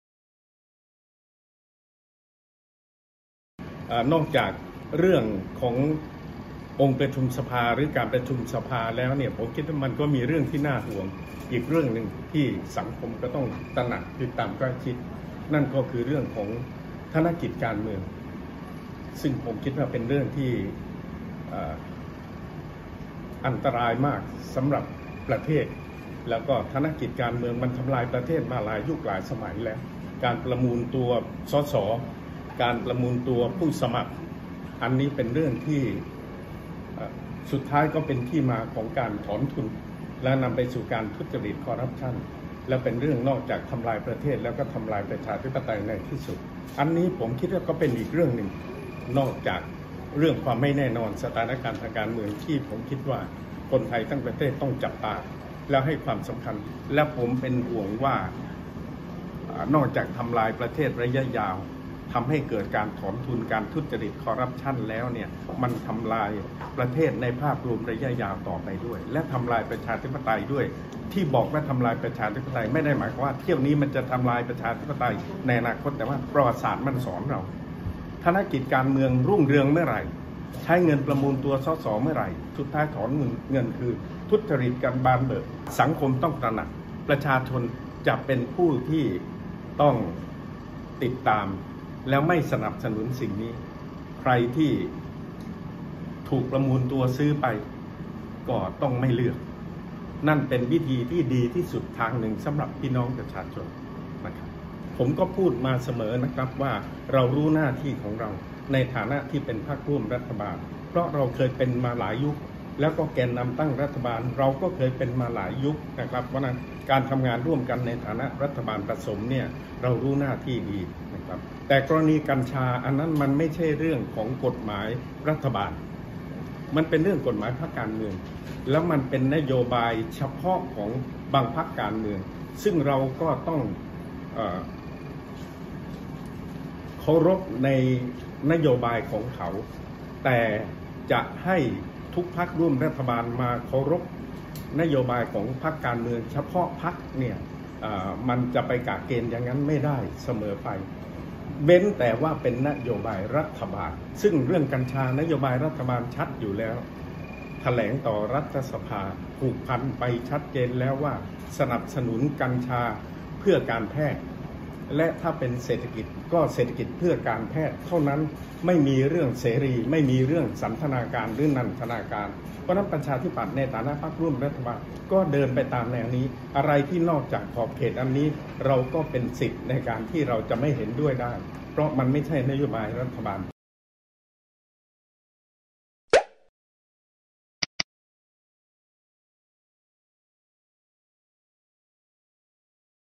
“จุรินทร์” รับห่วงประมูล ส.ส.ช่วงเลือกตั้ง ทำลาย ‘ปชต. : Matichon Online
“จุรินทร์” รับห่วงประมูล ส.ส.ช่วงเลือกตั้ง ทำลาย ‘ปชต.-ประเทศ’ ส่งเสริม ‘ทุจริตคอร์รัปชั่น’